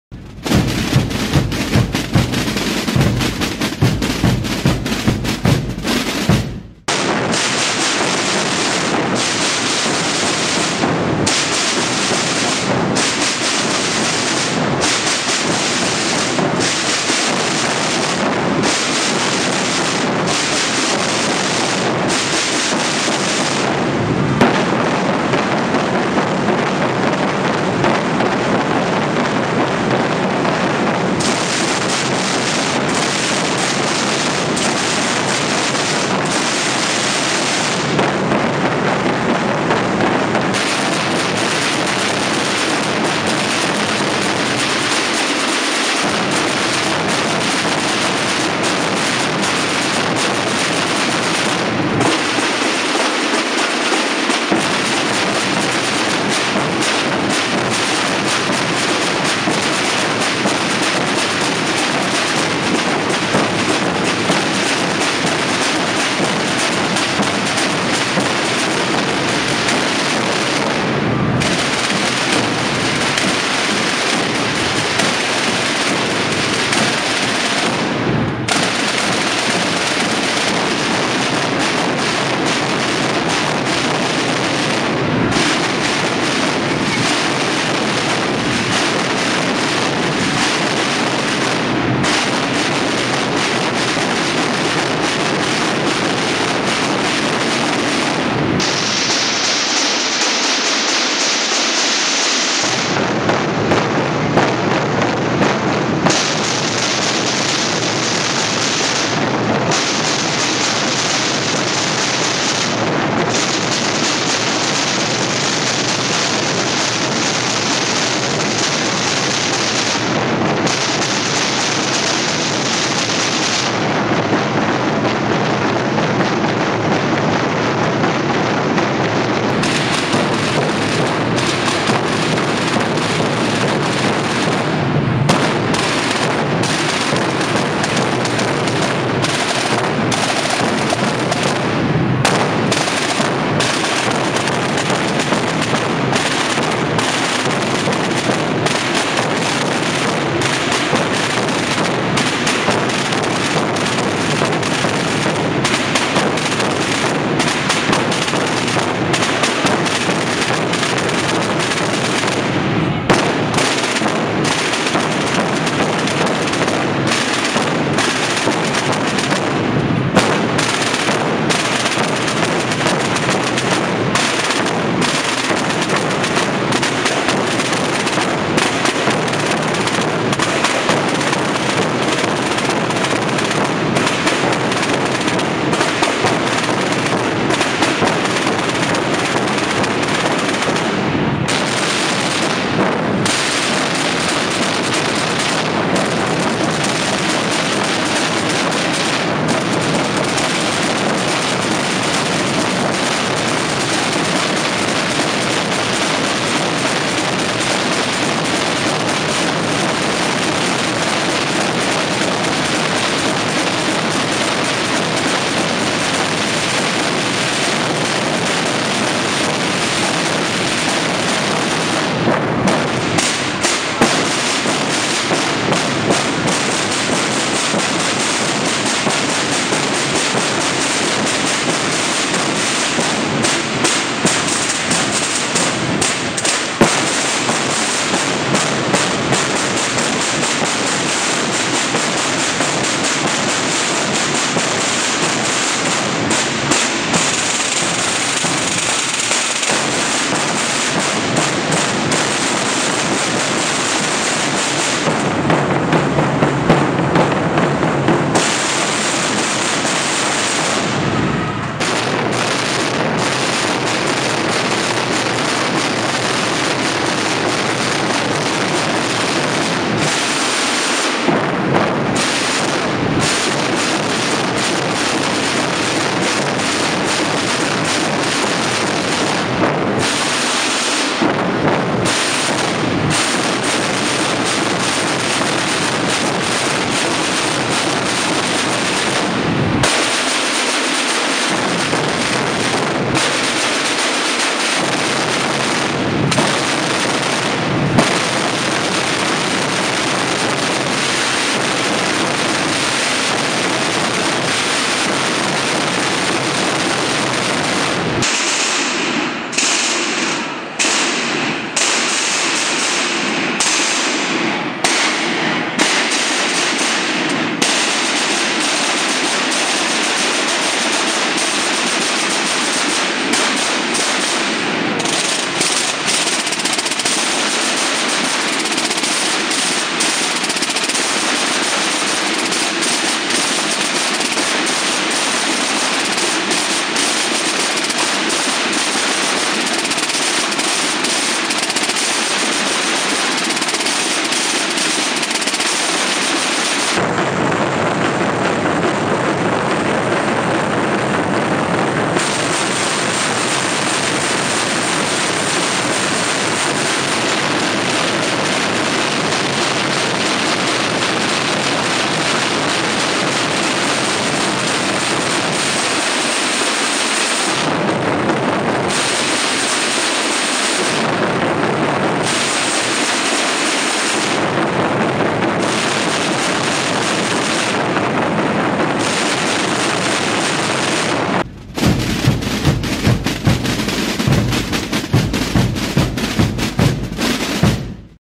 Exaltación Jornadas Ruta Tambor y Bombo en Alcorisa
Los nueve pueblos que forman la Ruta del Tambor y Bombo demostraron sus toques en la Exaltación que se celebró en Alcorisa con motivo de las Jornadas de Convivencia. El pabellón de fiestas de Alcorisa se quedó pequeño para escuchar los distintos ritmos de los tamborileros.